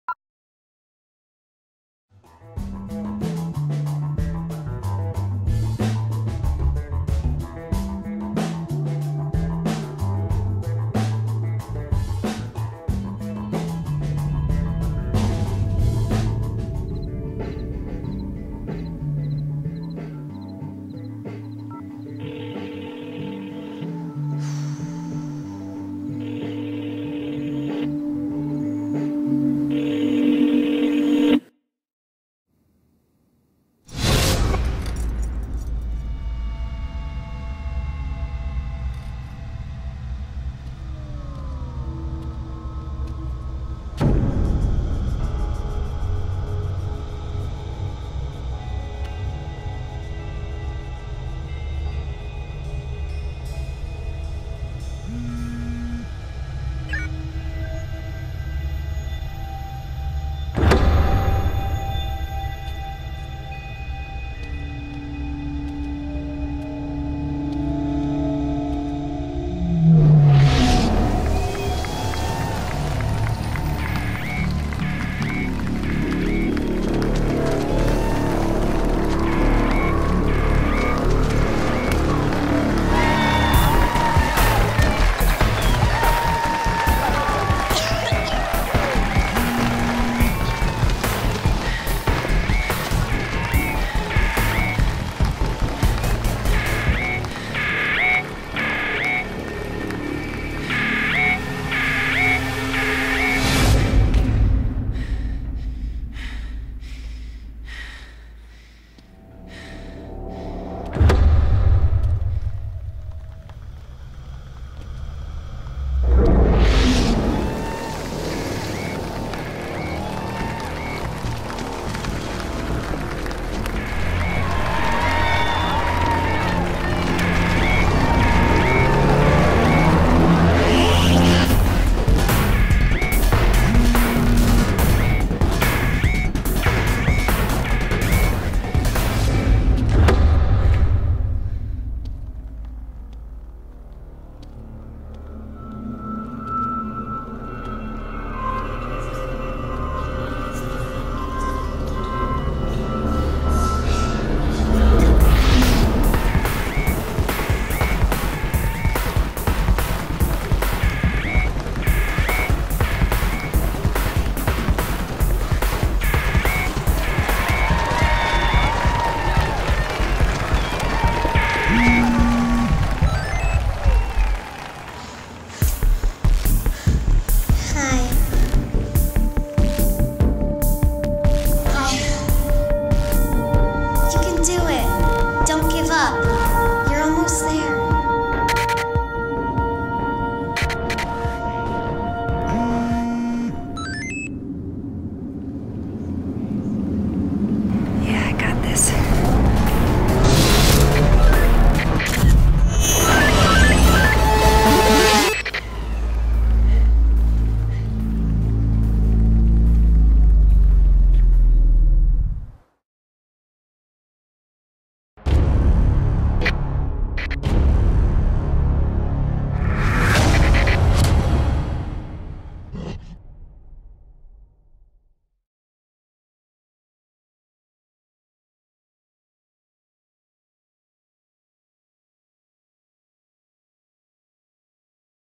Gin Wigmore as... KATELYN (4/5)
In 2014 Gin Wigmore did a 5 episodes commercial for Corning Gorilla Glass and htc.